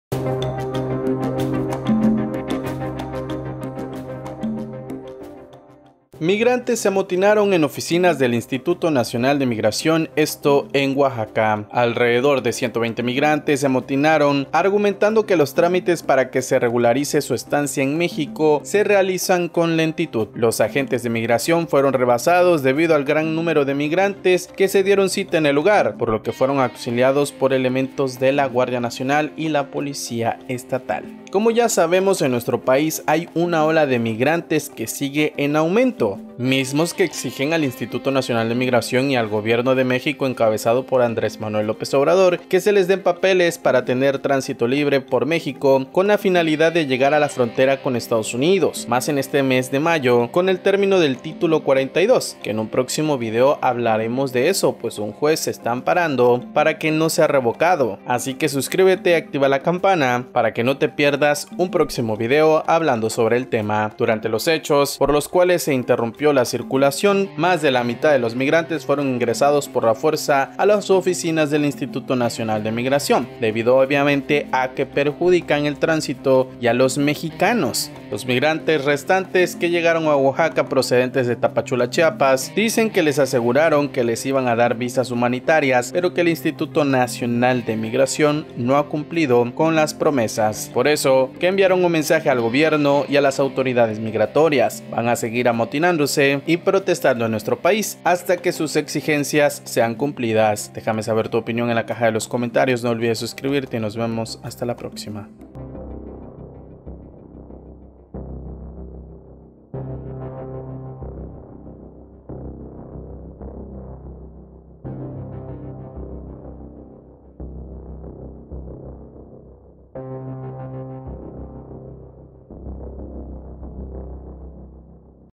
Migrantes En México: MIGRANTES se amotinan en estación del INM en Oaxaca; ¡EXIGEN visa humanitaria!
Alrededor de 120 migrantes se amotinaron en la subrepresentación local del Instituto Nacional de Migración (INM) de Oaxaca, argumentando que los trámites para que se regularice su estancia en México se realizan con lentitud.

Los agentes de migración fueron rebasados debido al gran número de migrantes que se dieron cita en el lugar, por lo que fueron auxiliados por elementos de la Guardia Nacional y de la Policía Estatal.

Durante los hechos, por los que se interrumpió la circulación sobre la carretera federal 17, más de la mitad de los migrantes fueron ingresados por la fuerza a las oficinas del INM